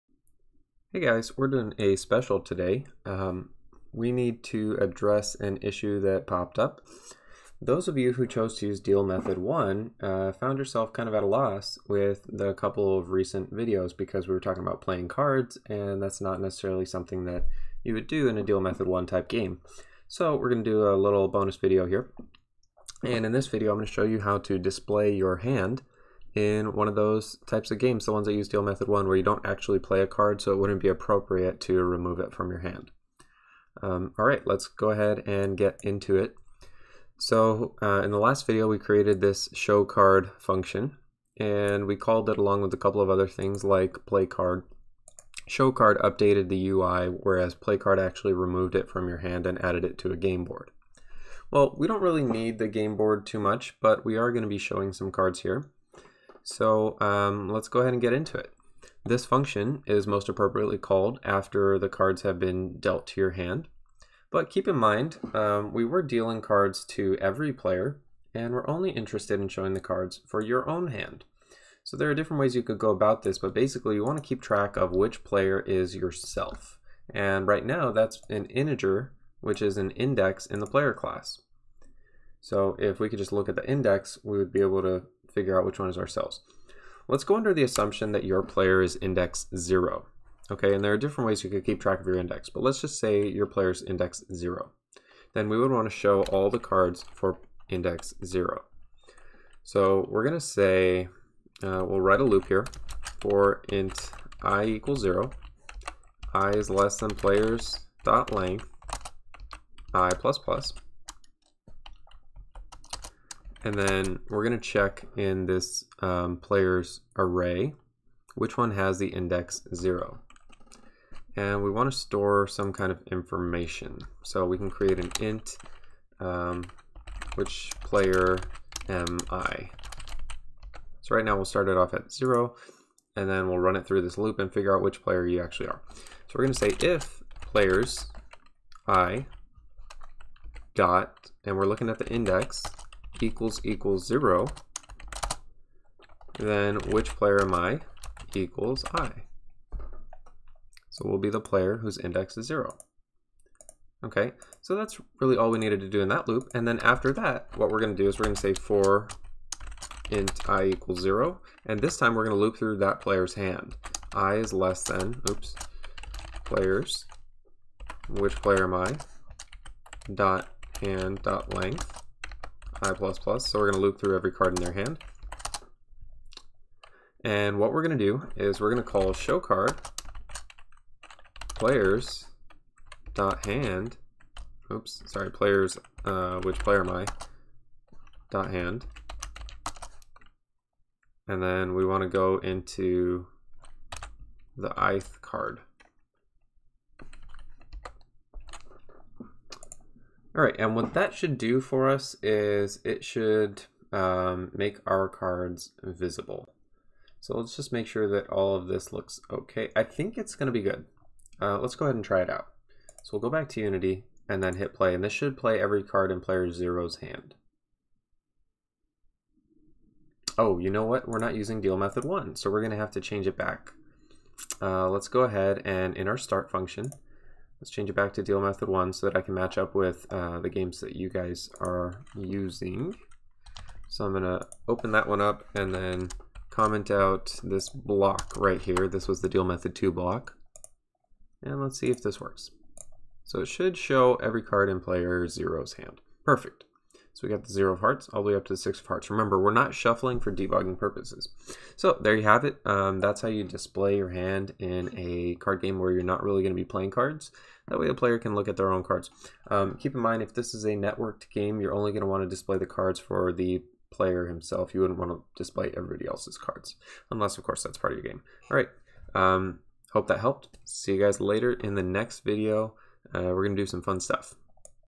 Card Game Tutorial Series (Video 10) DM1 Displaying the Hand
This video is for all the Deal Method 1 users out there from video 6 in this series. It can also be used in conjunction with playing a card to show the player's hand in a trading card game or other card game (with a couple small modifications). Even if you're not planning on using it, check out the video to get a feel for how to make small changes like this.